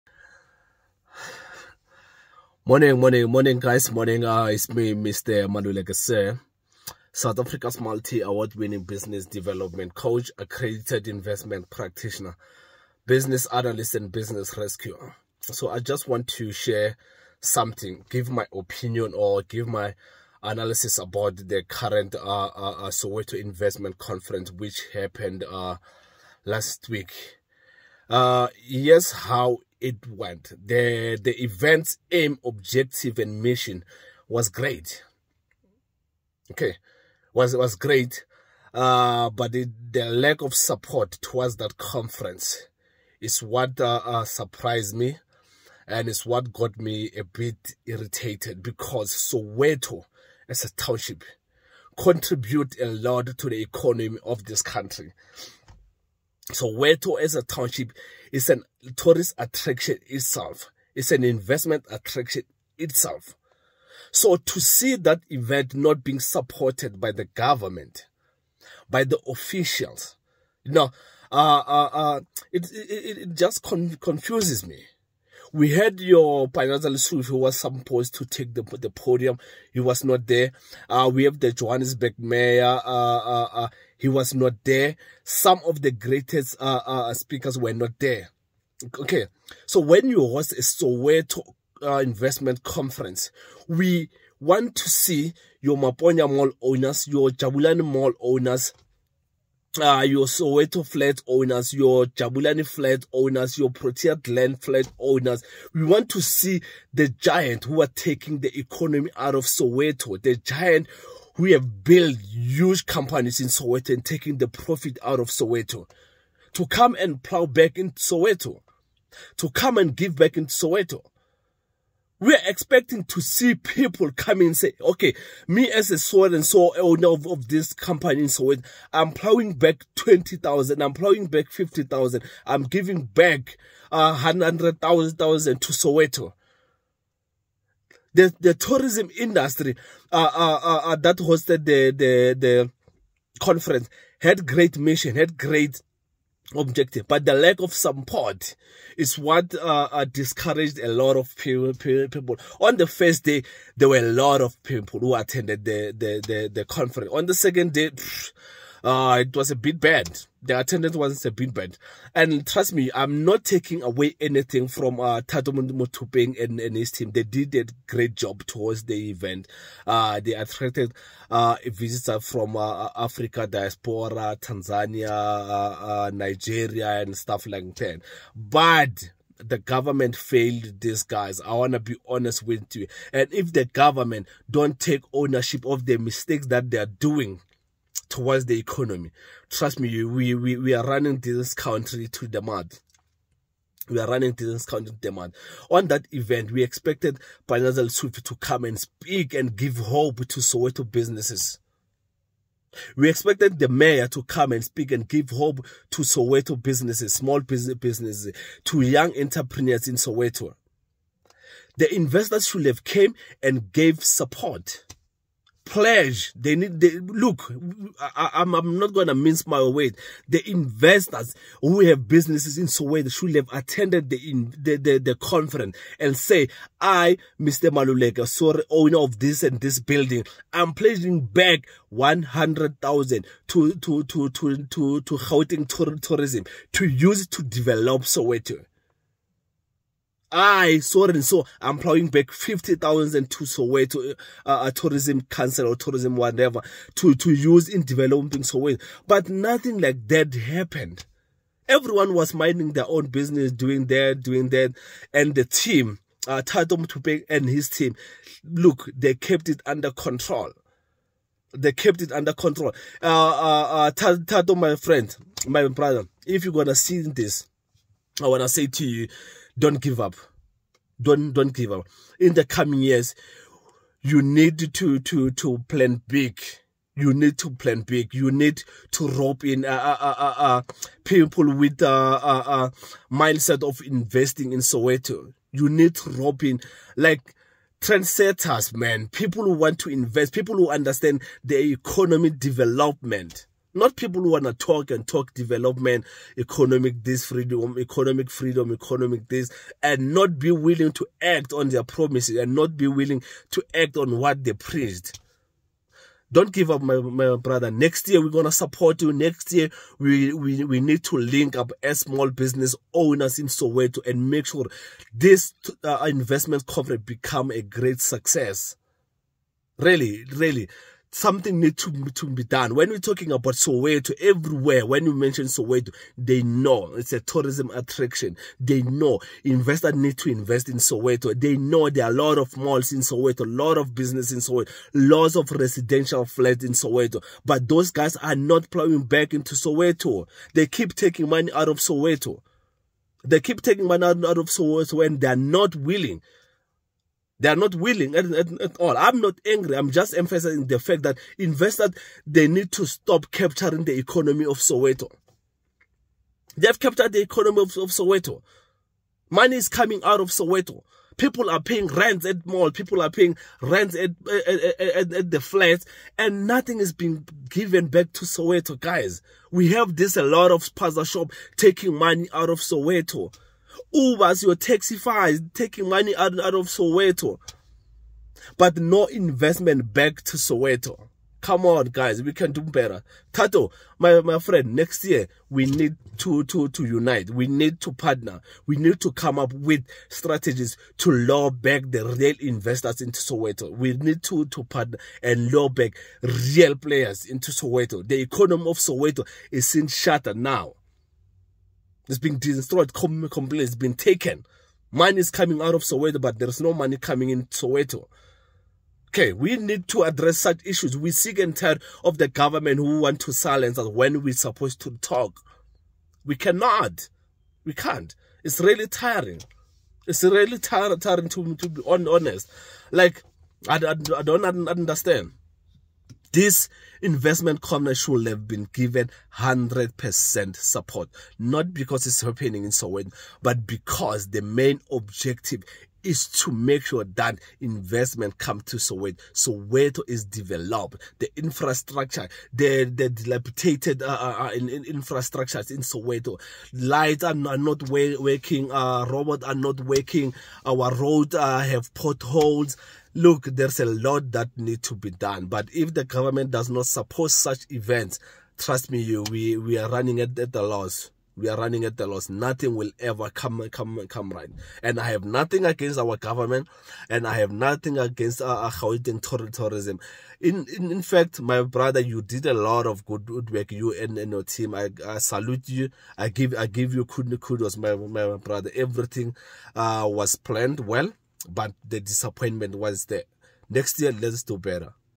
Soweto Investment Conference lacked support
South Africa's Multi Award Winning Business Coach  | Accredited Investment Practitioner | Business Analyst and Rescuer MR MALULEKE SIR addressing the lack of Support by government towards Soweto Investment Conference 2023